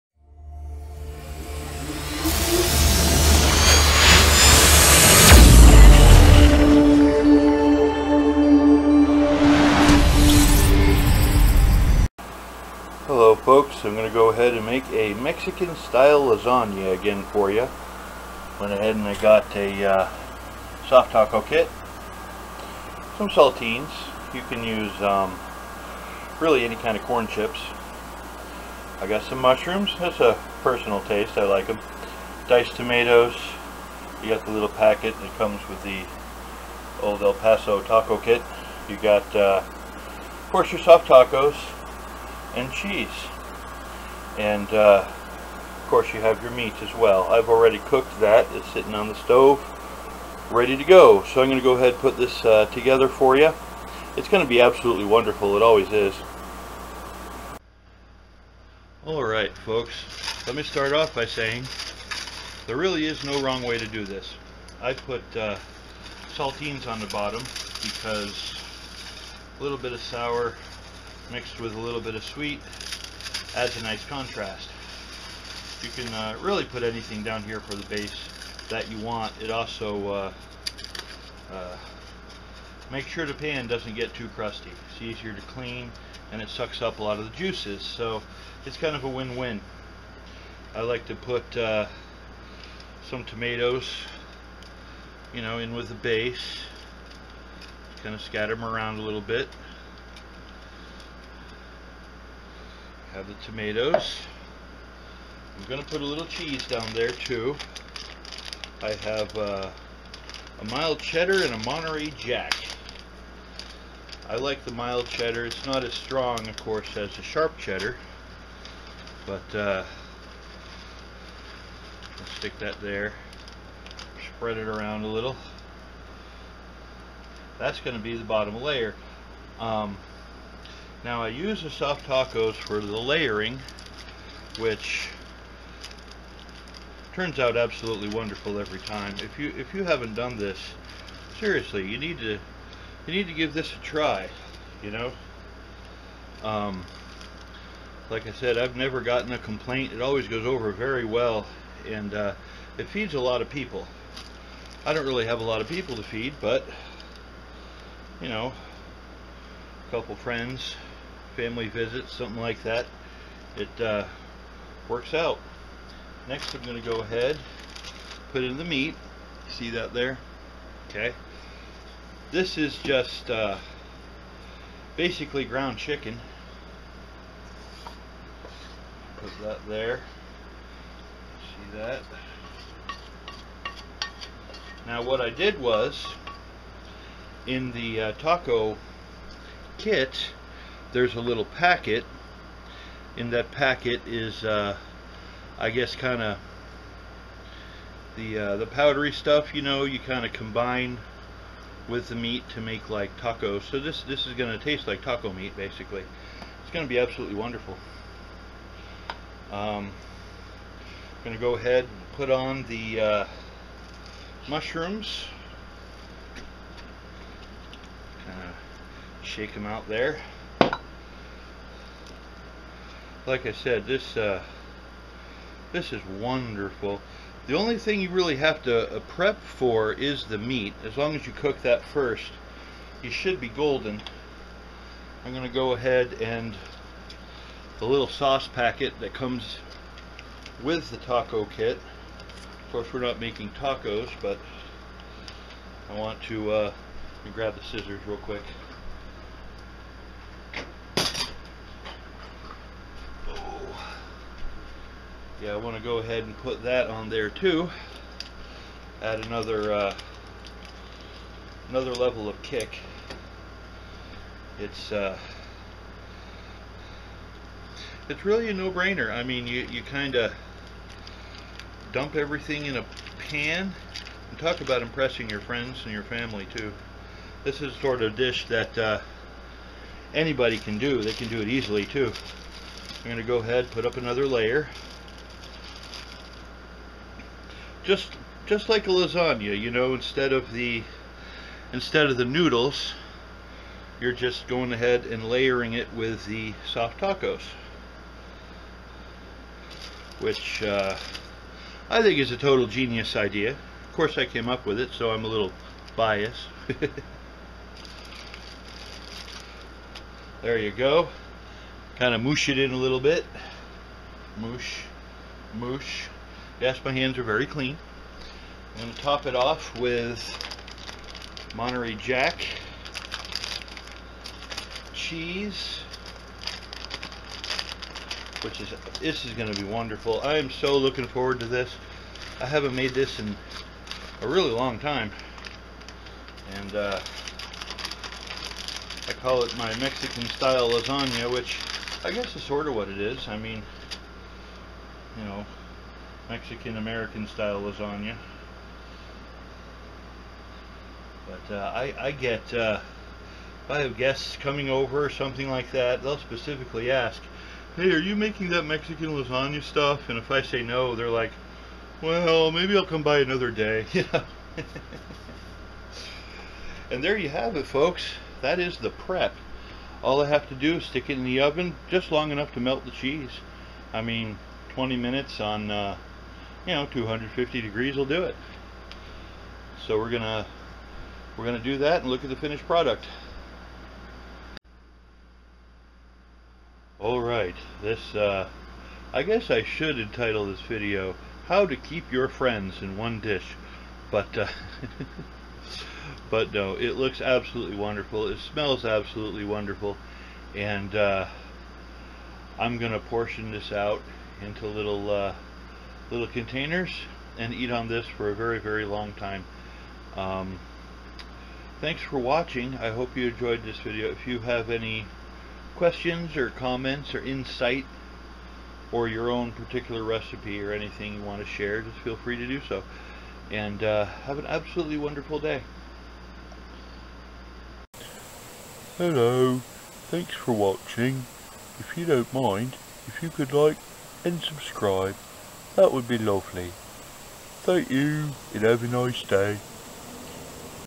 Mexican Taco Inspired Lasagna
I'm not really a hard core foodie but I do like coking from time to time!  Thanks for stopping by and watching; Mexican Taco Inspired Lasagna!